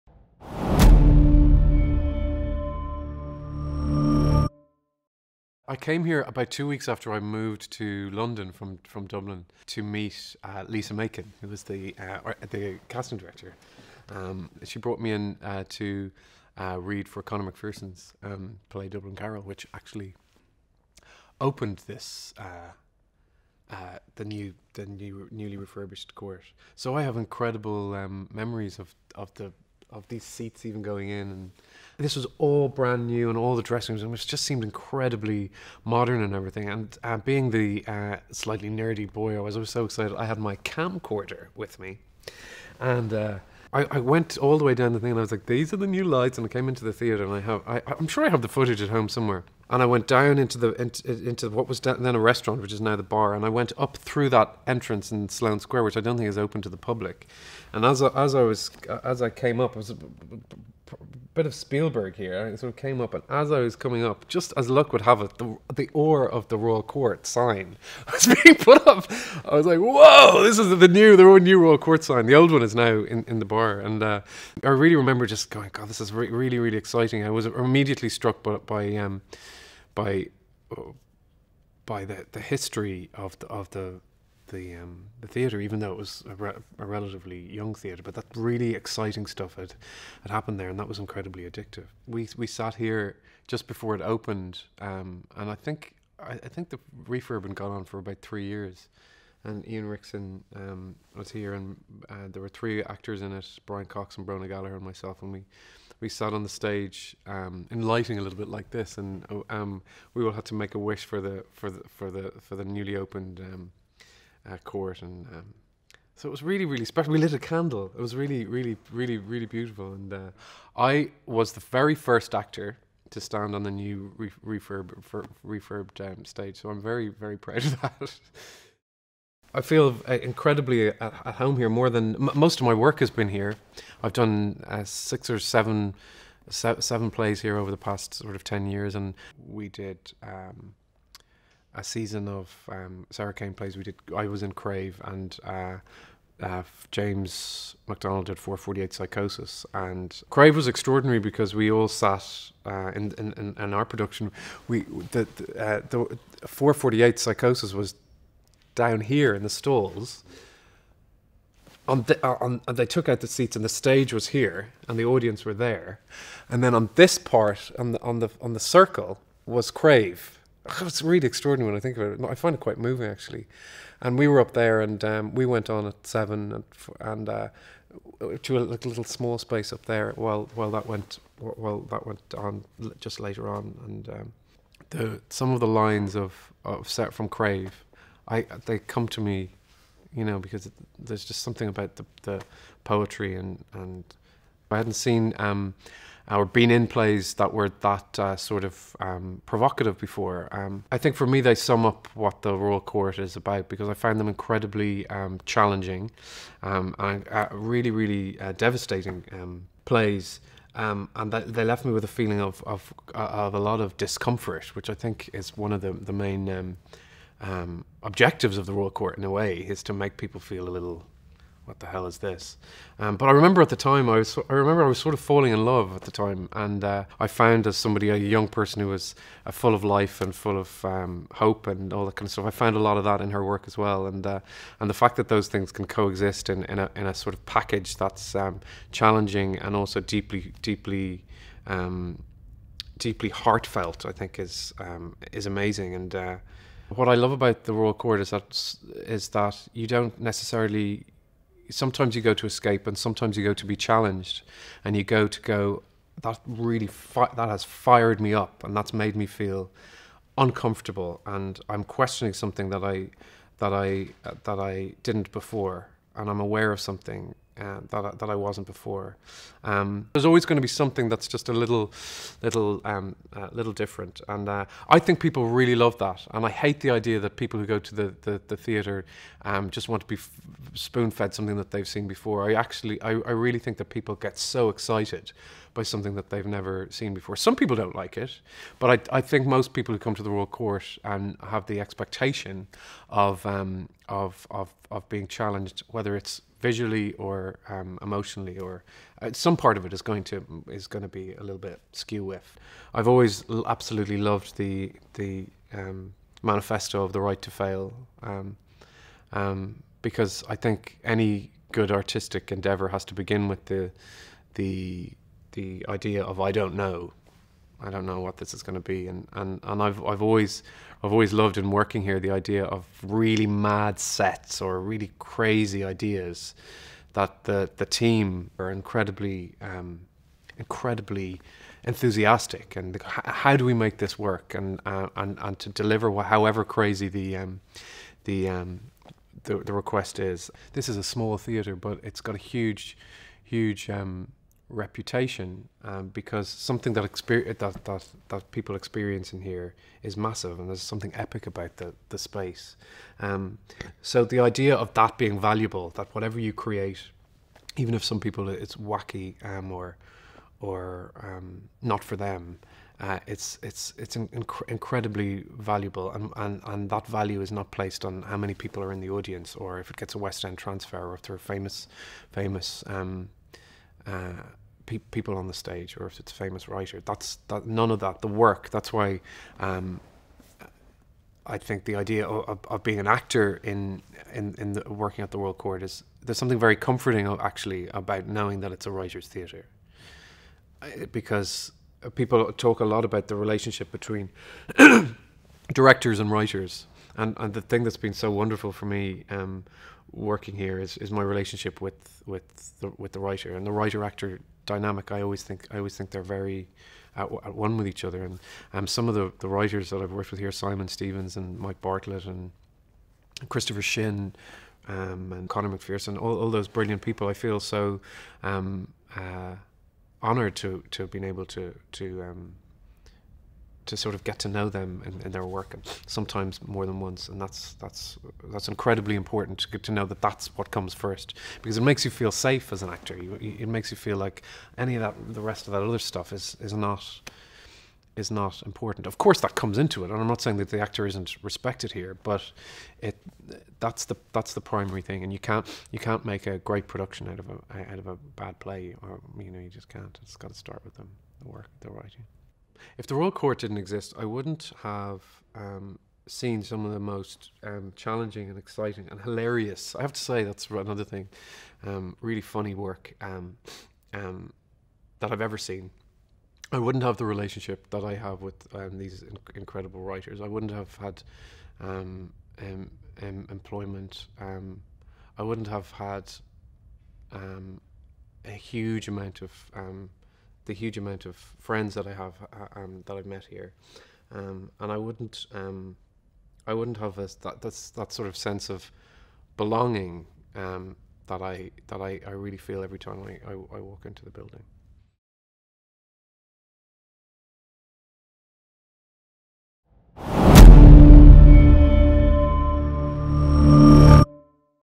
Andrew Scott on the Royal Court Theatre
Andrew Scott  discusses the Royal Court Theatre at 60 years.